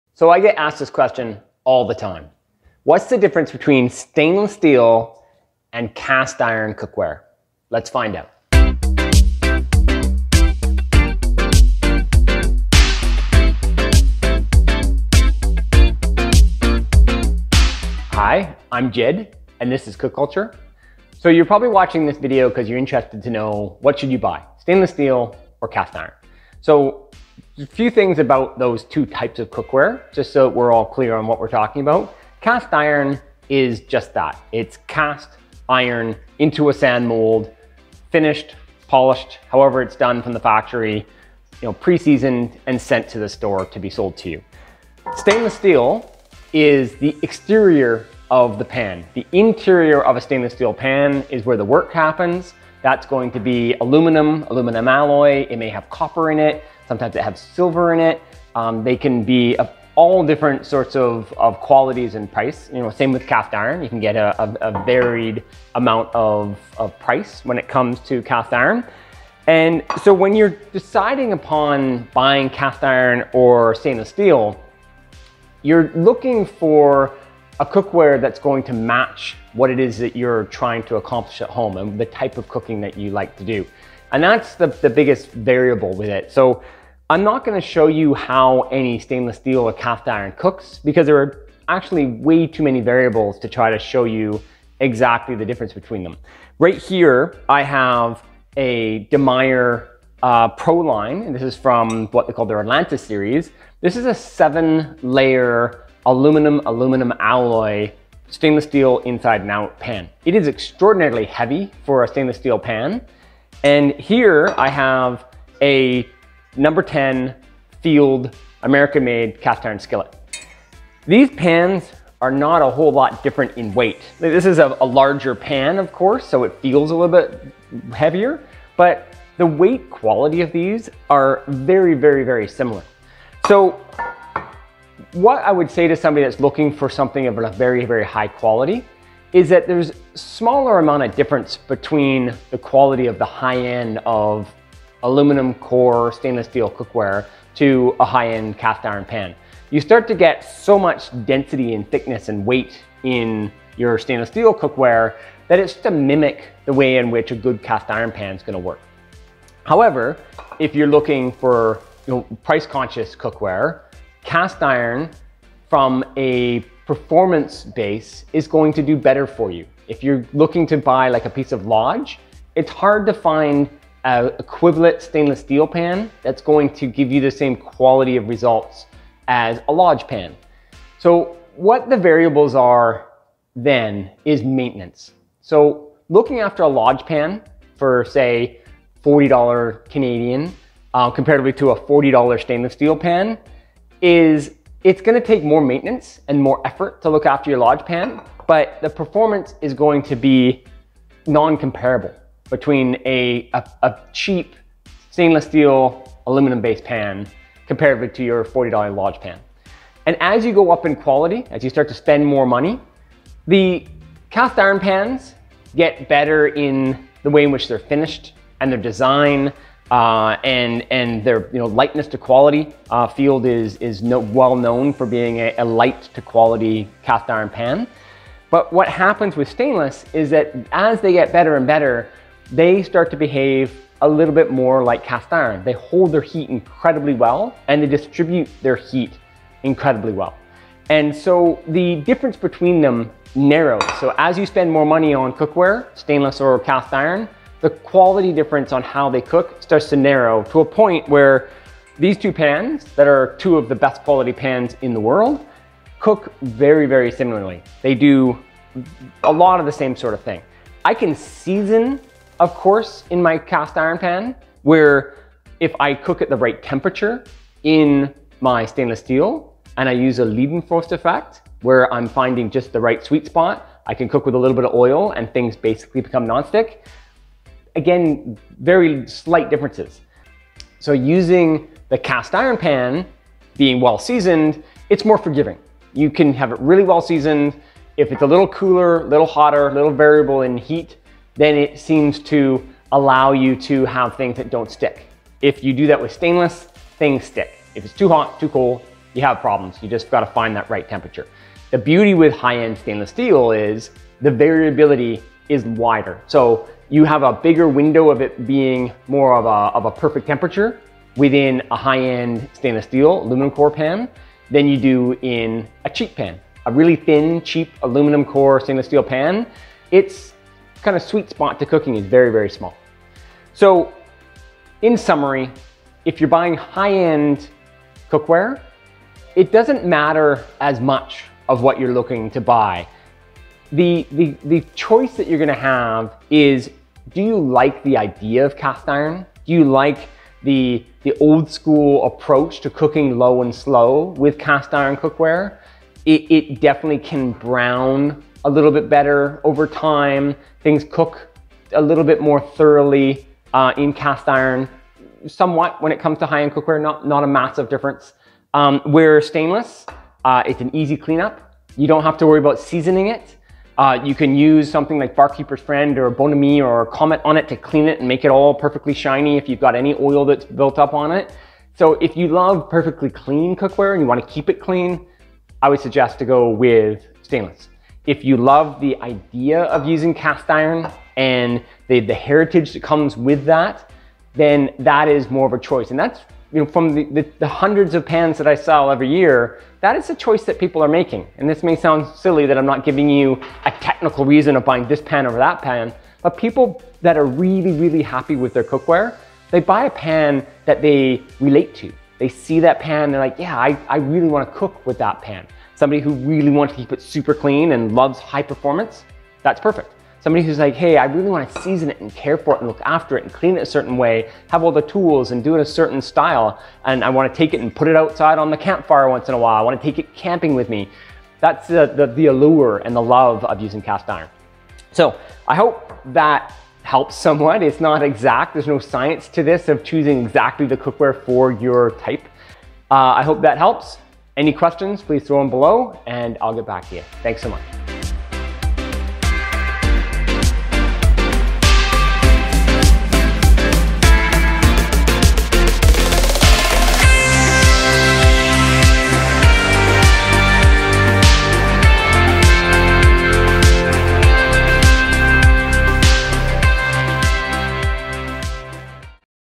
Stainless Steel Frying Pan vs Cast Iron Skillets? What's best for you!
I have been asked this a 1000 times - "what should I buy, a Stainless Steel Frying pan or Cast Iron Skillet?"

I address that question in this video and attempt to simplify the answer so that you can more confidently but the cookware that will work best for you.